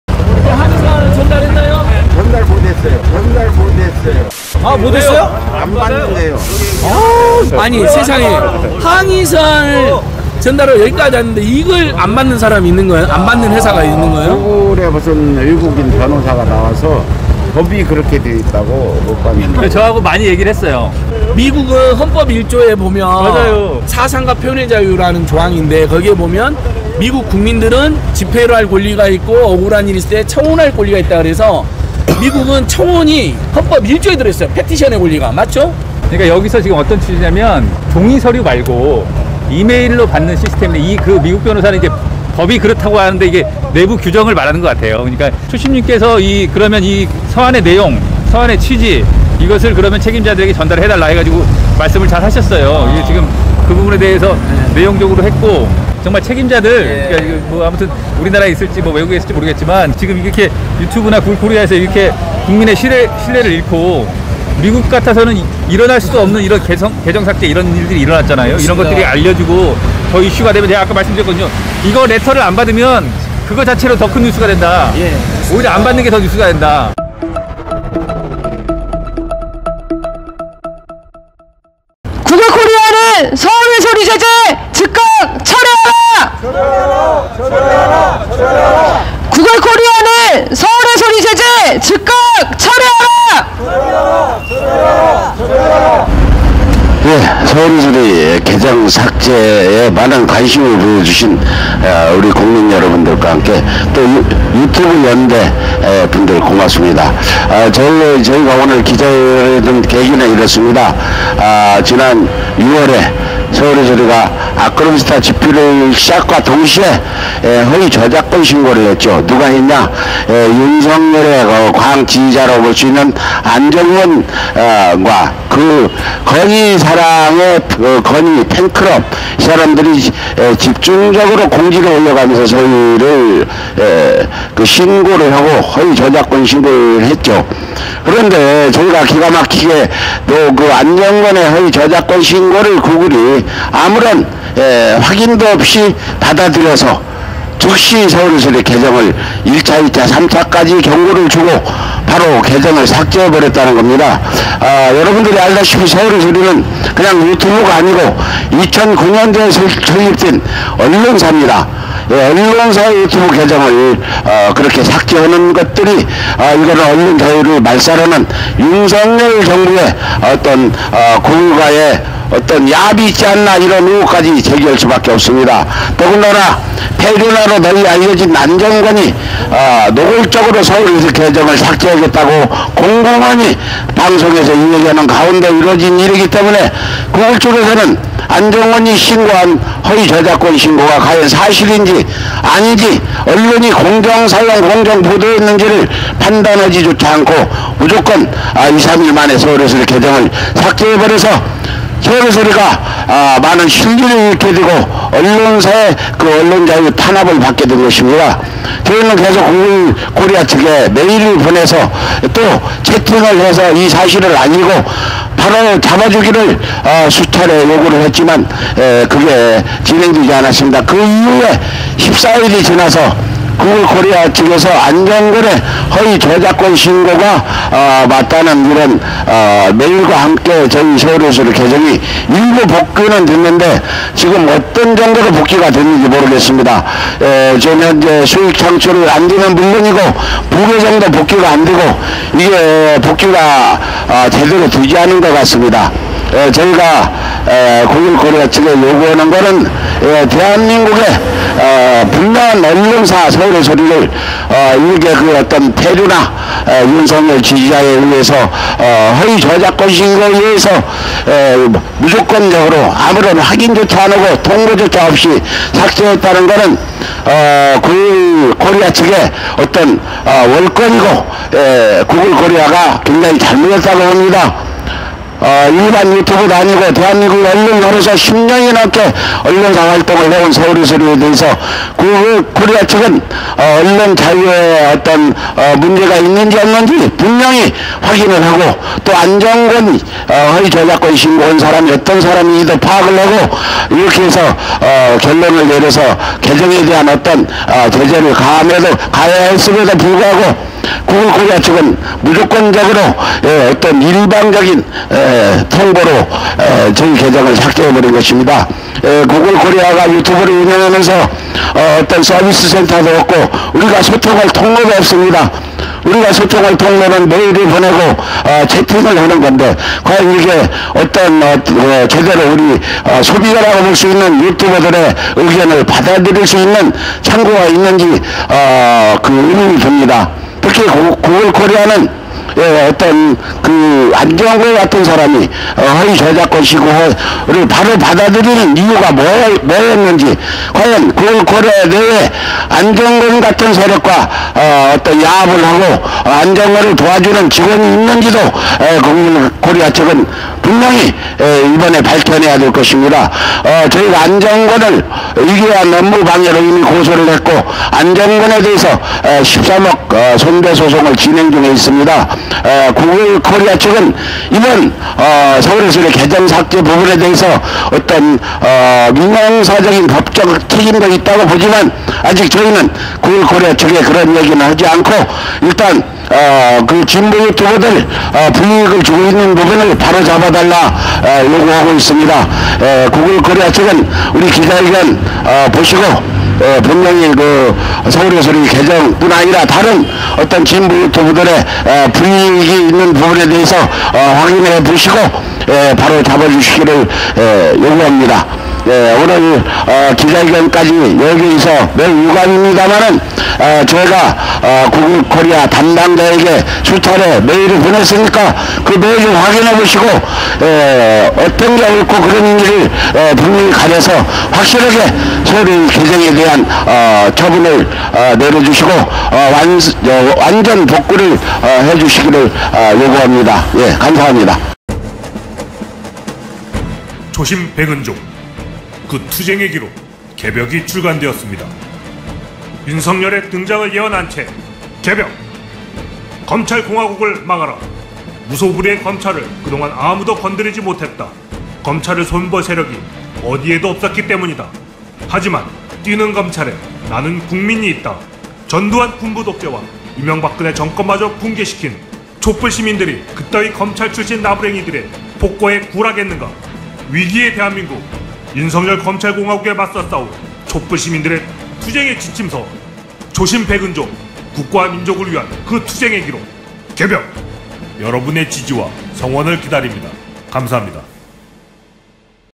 항의서한 거부한 구글코리아! "안 받는 것이 더 큰 뉴스가 된다!"
서울의소리가 윤석열 폭정에 맞서 싸울수  있도록 성원을 호소합니다.